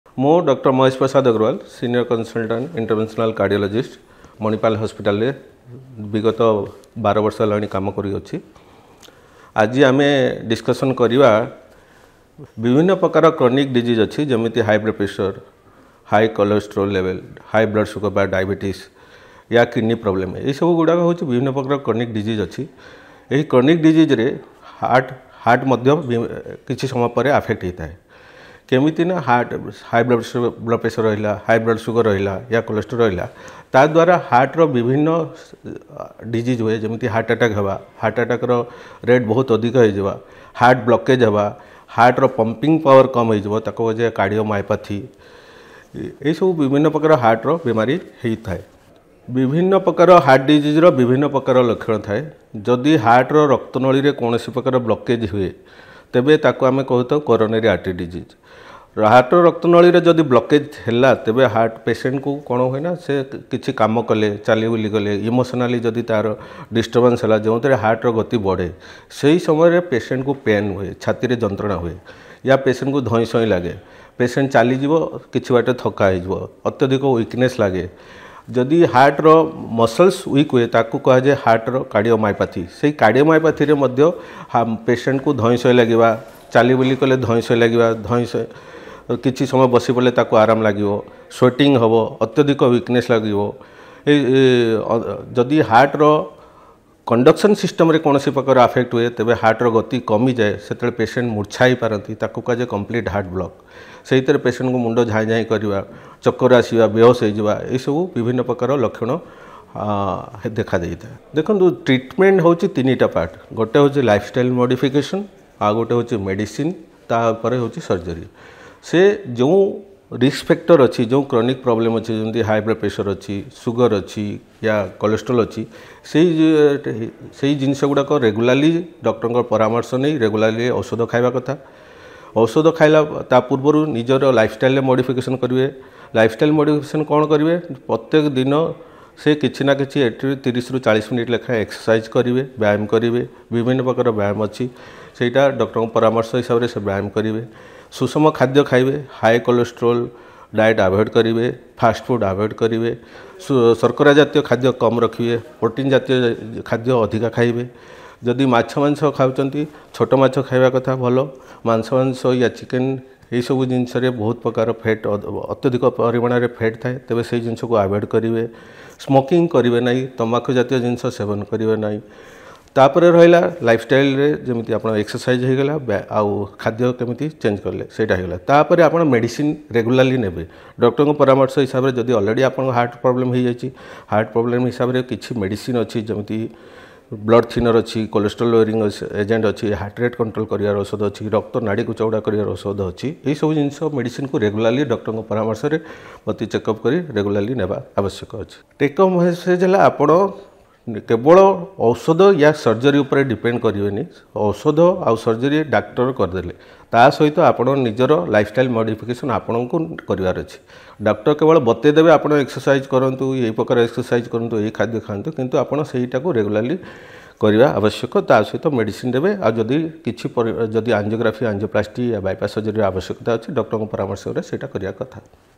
Chronic Illnesses and Heart Disease - Dr. Mahesh Prasad Agrawala | Manipal Hospital Bhubaneswar
In this video, Dr. Mahesh Prasad Agrawala, Consultant - Interventional Cardiology at Manipal Hospital, Bhubaneswar, explains how chronic conditions like diabetes, obesity, and high blood pressure can strain your heart. He discusses the link between chronic illnesses and heart disease, highlights early warning signs you should never ignore, and shares effective lifestyle changes and medical tips to safeguard your heart health.